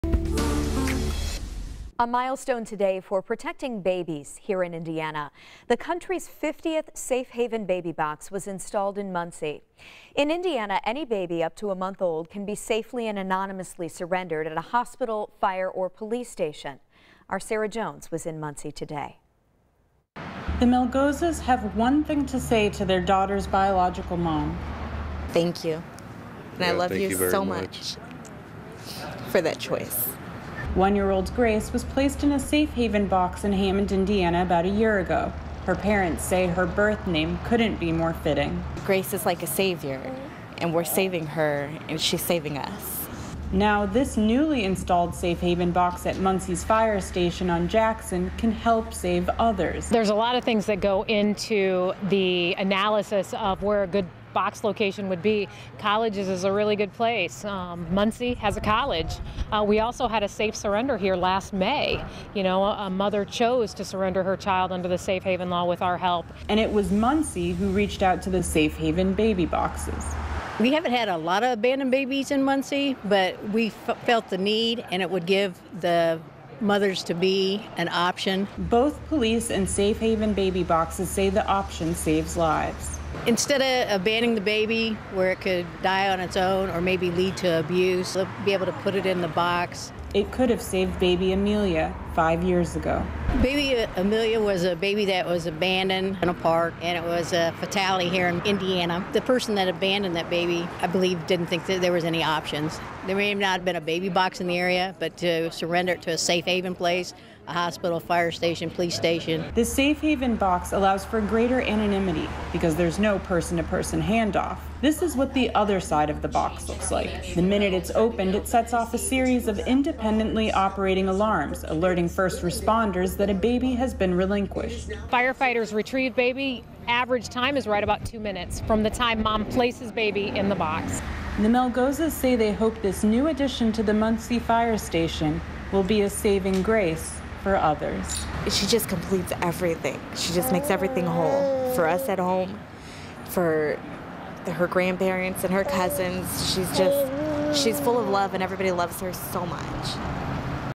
Safe Haven baby box installed in Muncie
The country's 50th Safe Haven Baby Box was installed in Muncie.